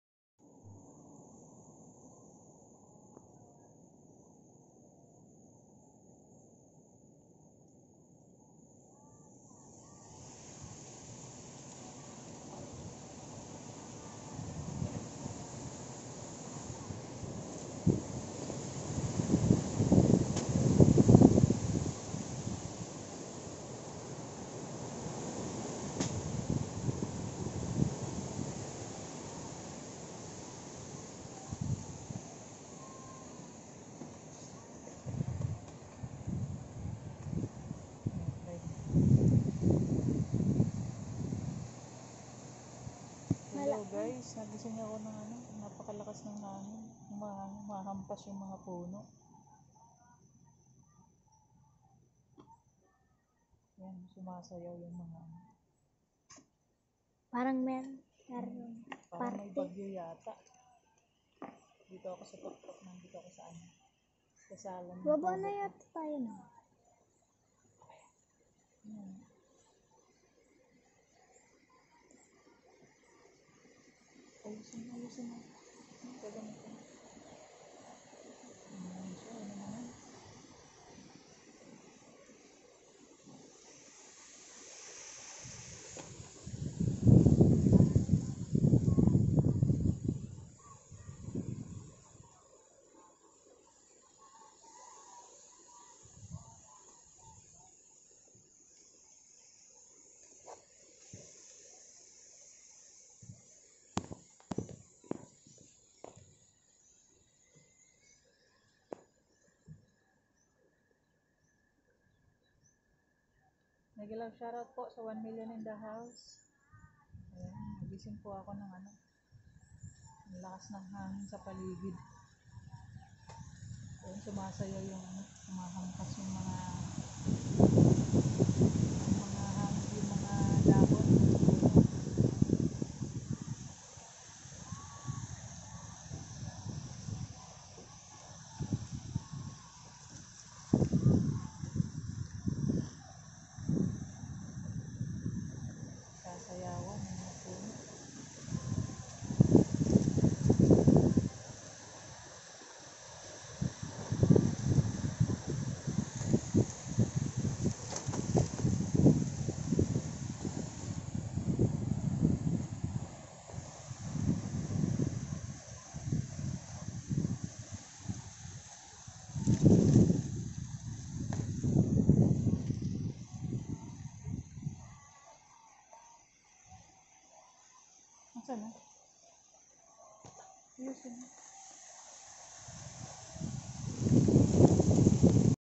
NATURE IS SWAYING
I woke up with those trees been swaying and dancing. A windy morning that is. Due to some LLPA coming. And the sounds of nature is just amazing.